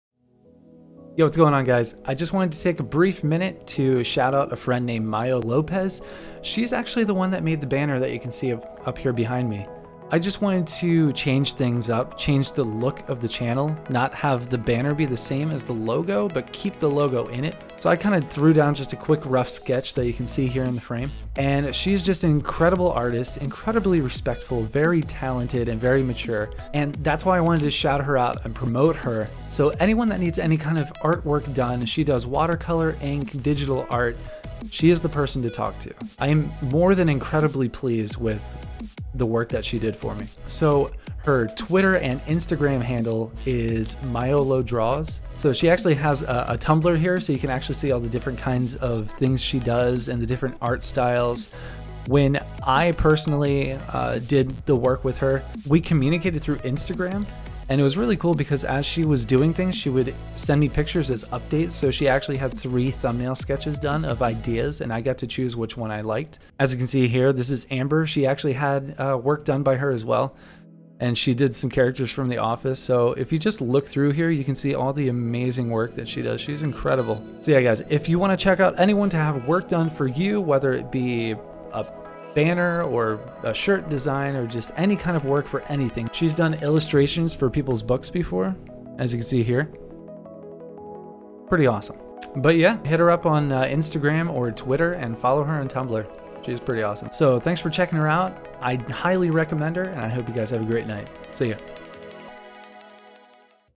MAYO LOPEZ| CREATOR OF MY NEW BANNER ART
Hey guys, I just would like you to take a minute to check out Mayo Lopez. She is an incredible artist and she's the one that created my new banner art for the channel. If you need any kind of digital, ink, or watercolor artwork done for you; consider Mayo.

Mayo's Twitter & Instagram: @Mayolodraws